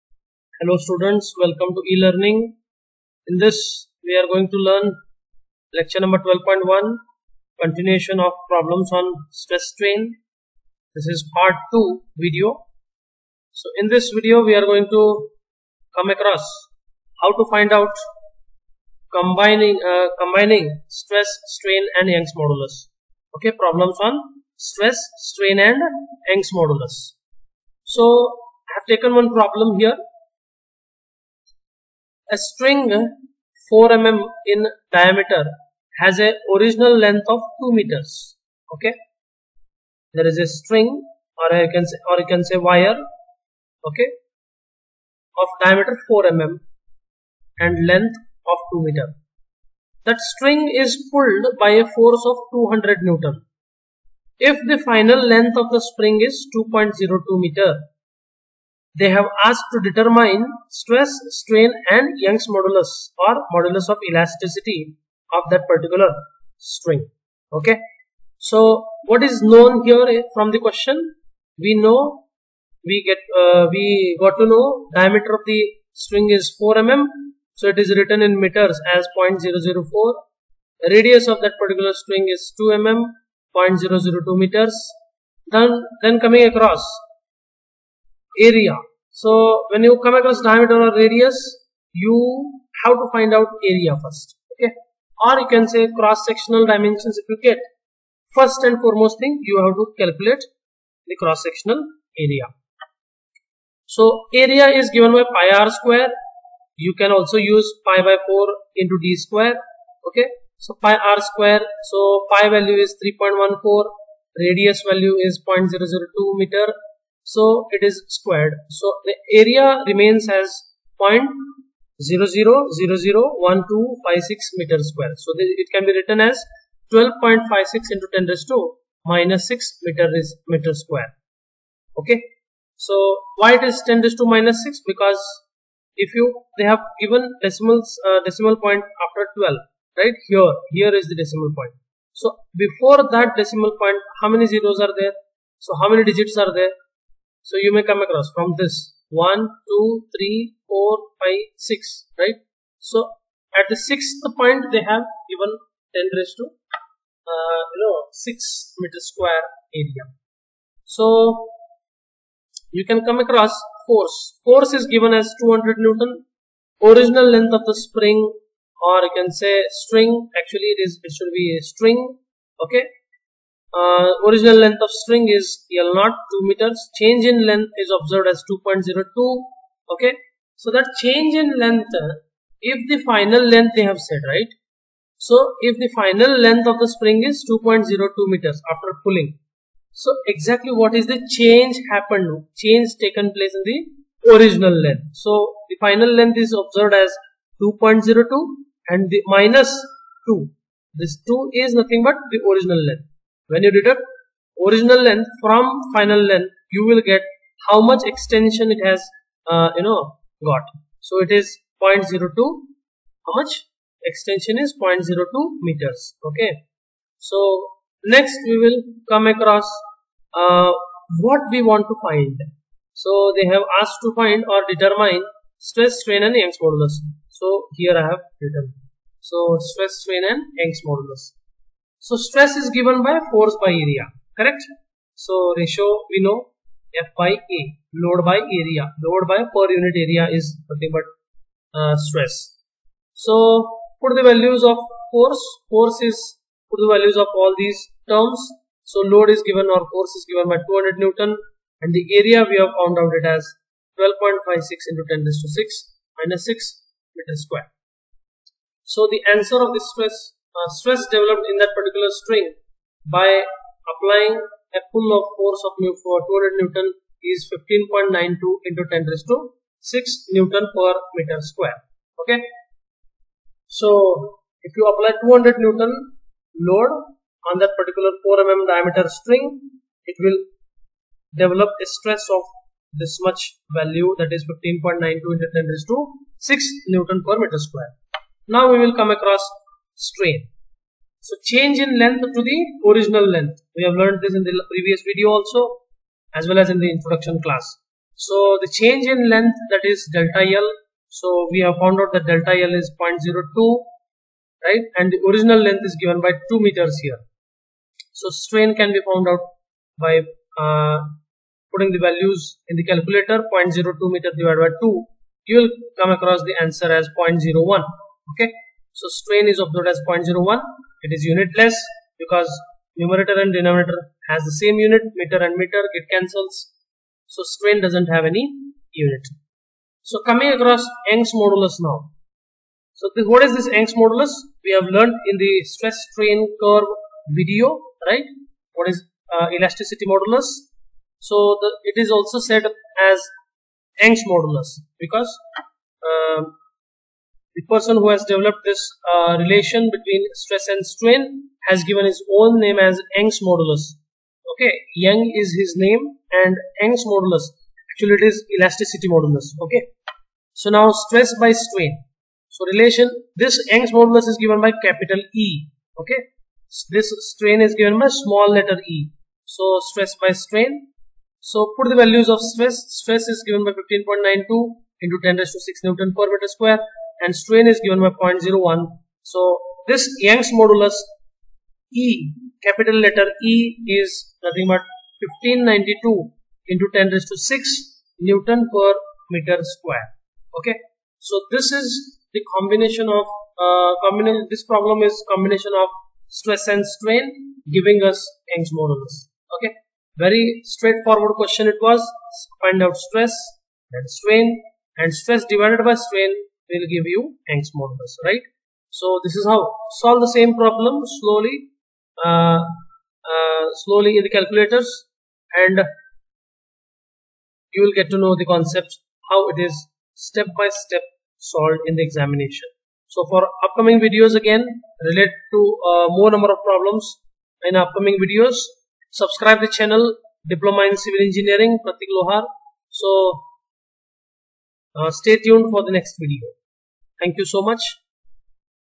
15CE31T Problems on Stress Strain Young's Modulus (Modulus of Elasticity) Lecture 12.1
Problems on Stress Strain Young's Modulus (Modulus of Elasticity)
Step by Step calculations